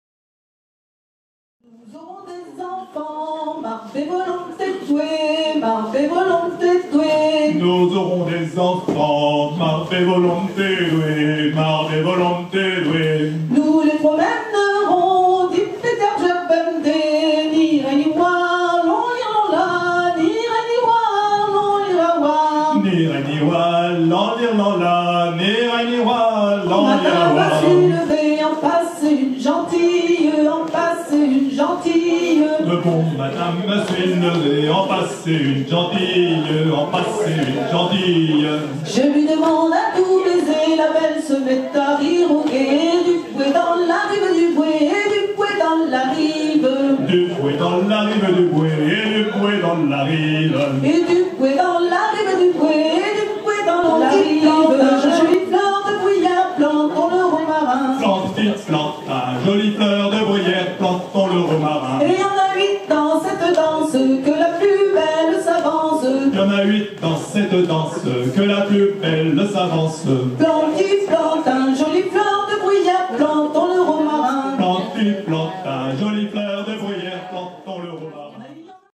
Cercle Celtique de Vertou - Bal Breton de l'Avent 2022 : Duo Dérédec
Prestation du Duo Dérédec au Fest-Deiz 2022 du Cercle Celtique de Vertou